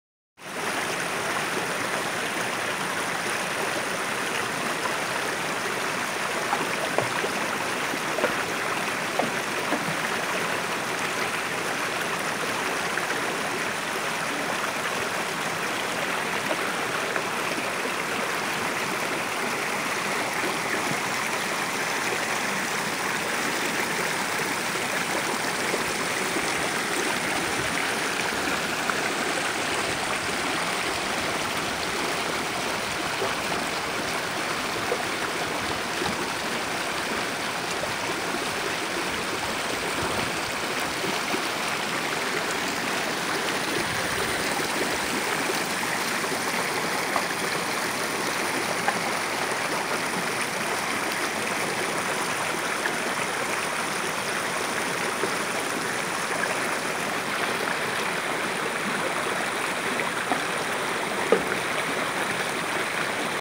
*Gorgeous Sparkling Sunshine On the Water - Mount Shasta* Sparkling Gems in the Sunshine
My daughter and I went to Mount Shasta over the weekend and we discovered this gorgeous sparkling mountain stream...11/11/2019.   💗💗💗

Note:  my daughter was throwing stones into the water...those were not fish rising!